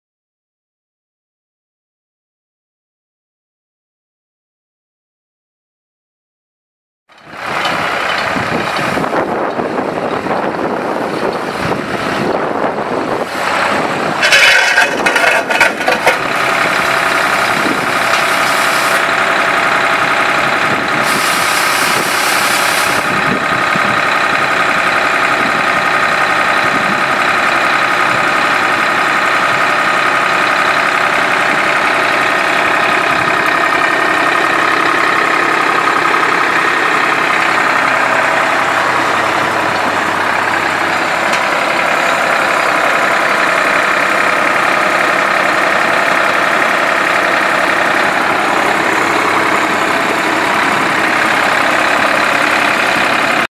1996 New Holland 3930 tractor for sale | sold at auction June 30, 2015
SOLD! City of Wichita Surplus Auction

1996 New Holland 3930 tractor
1,225 hours on meter
New Holland three cylinder diesel engine
Serial BB647908-6K07
Manual transmission
8F-2R gears
Two post roll bar
Shop built canopy
Plywood roof damage
Kurb-Kutter 700-L attachment
Three level hydraulic drive
Cutter disk
Wire brush broom
Serial 98-525
Three point
540 PTO
84.5" wheelbase
7.50-16SL front tires
14.9-24 rear tires
Serial 047718B
Unit# 50082